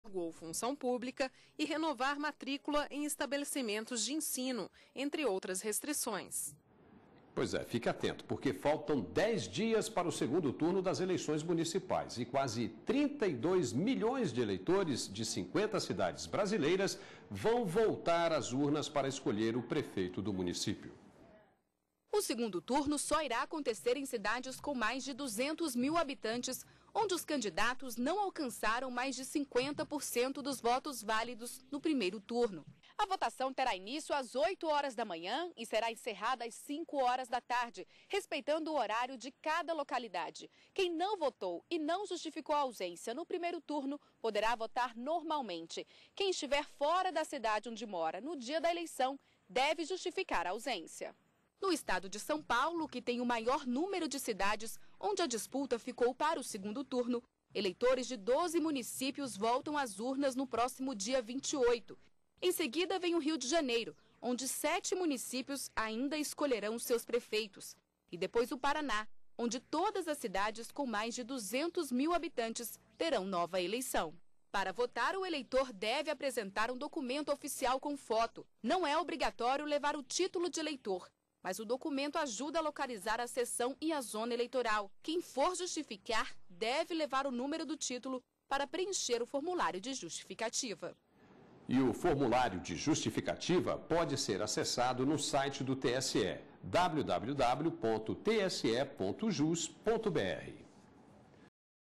Segundo turno eleições 2012 (18/10/12)
Faltam dez dias para o segundo turno das eleições municipais. Quase 32 milhões de eleitores de 50 cidades brasileiras vão voltar às urnas para escolher o prefeito do município.
O formulário de justificativa pode ser acessado no site do TSE, O eleitor também pode consultar o local de votação no mesmo endereço.